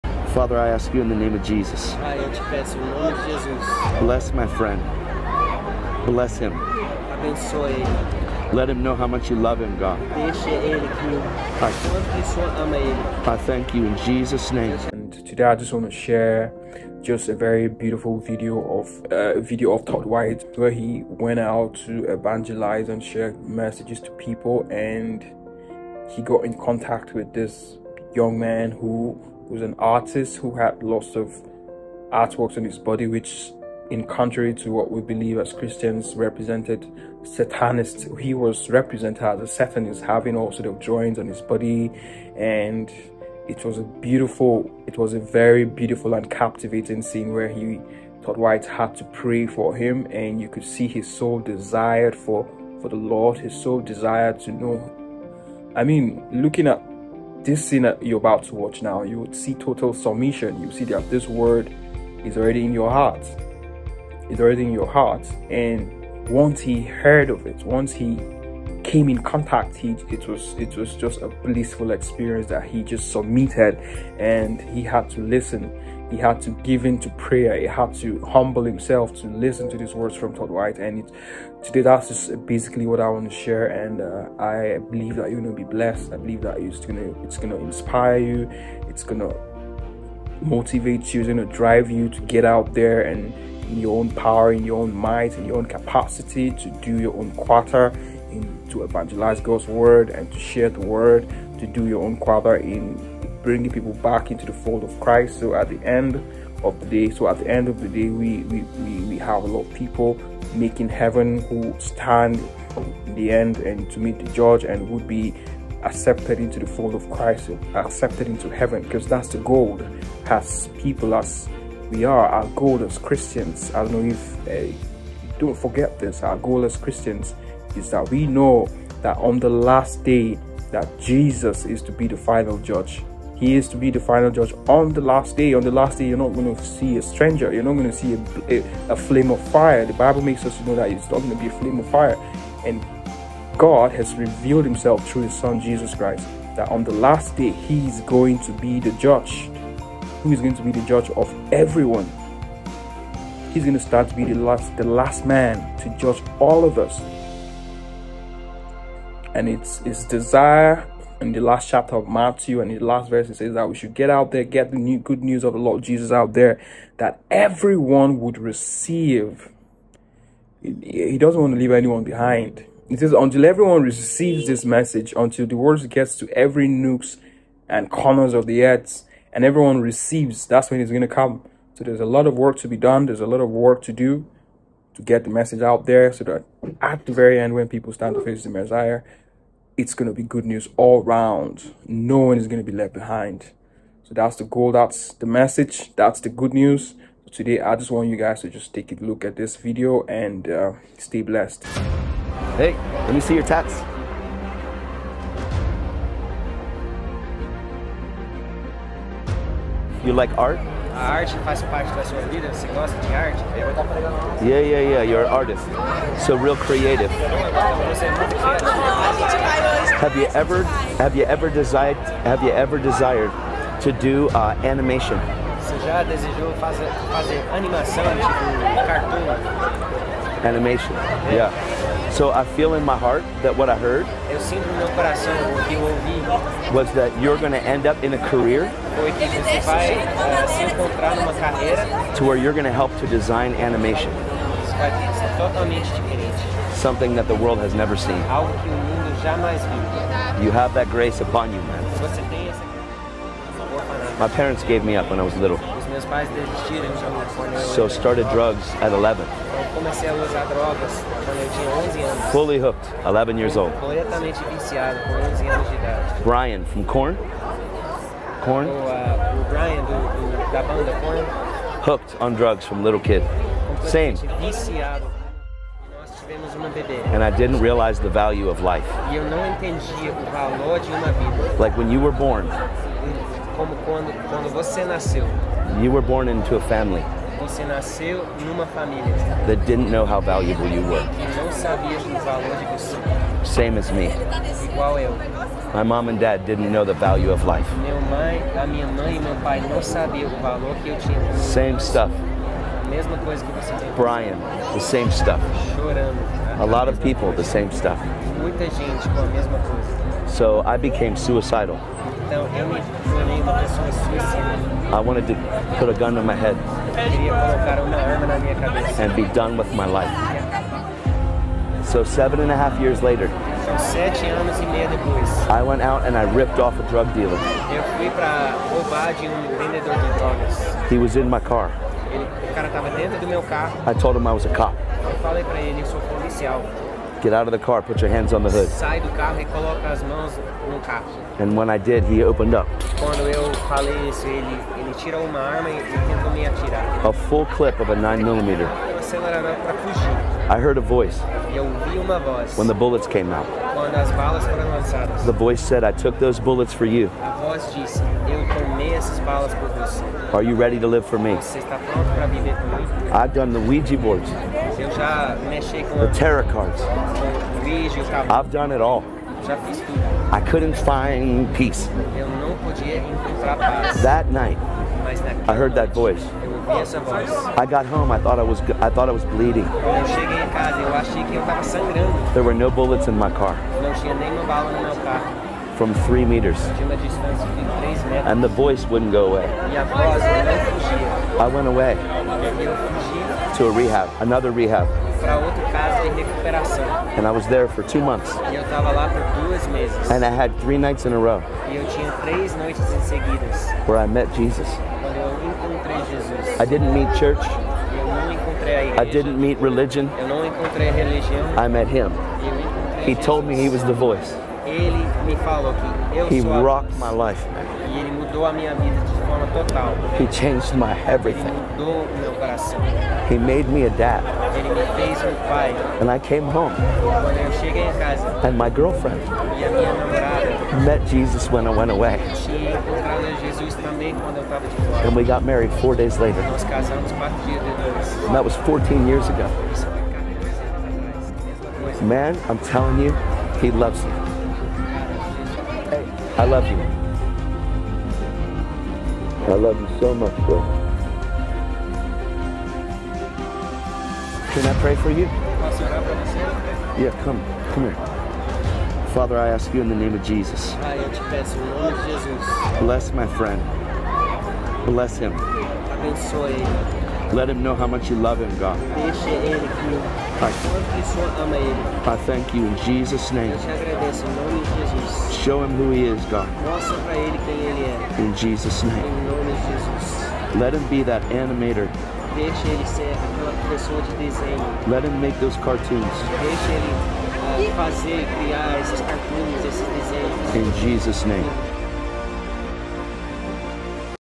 SATANIST ARTIST SUMMITS TO JESUS
Todd white captured the heart of a carnal Artist during his street Evangelism. This was a moment of brilliance and total submission to God.